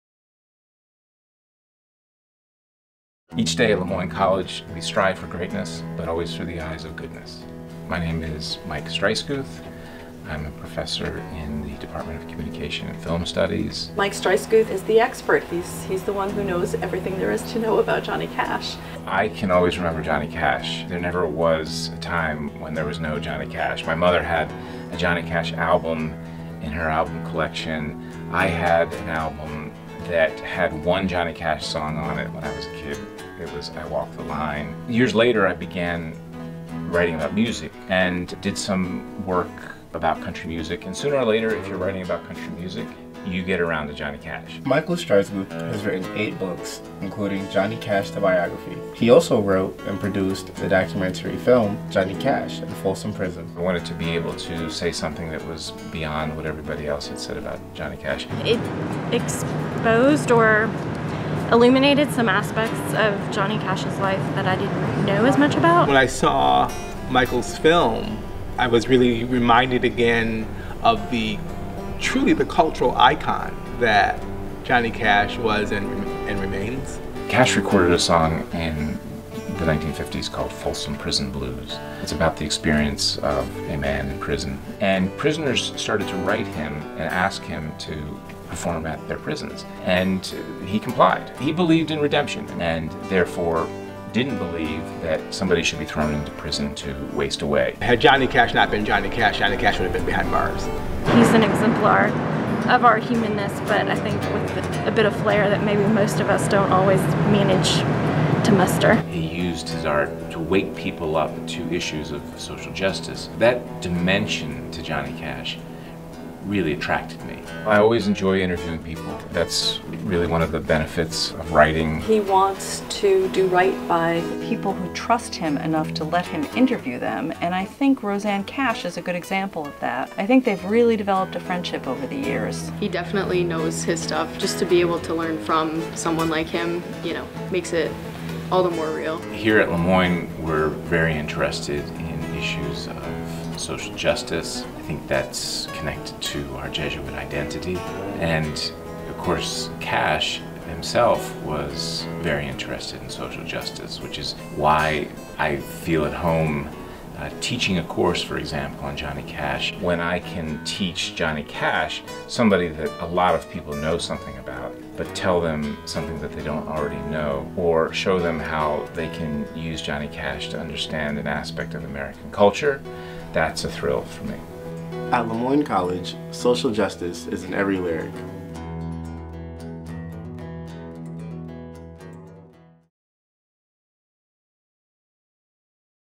Le Moyne College's Michael Streissguth
Most people know the late musician Johnny Cash as an artist and cultural icon – and he was. But Professor of Communication and Film Studies Michael Streissguth, who has written three books and a documentary on Cash, also knows him as a fierce advocate for social justice. That is a side of Cash that Streissguth is uniquely qualified to share with the world – and his students at Le Moyne.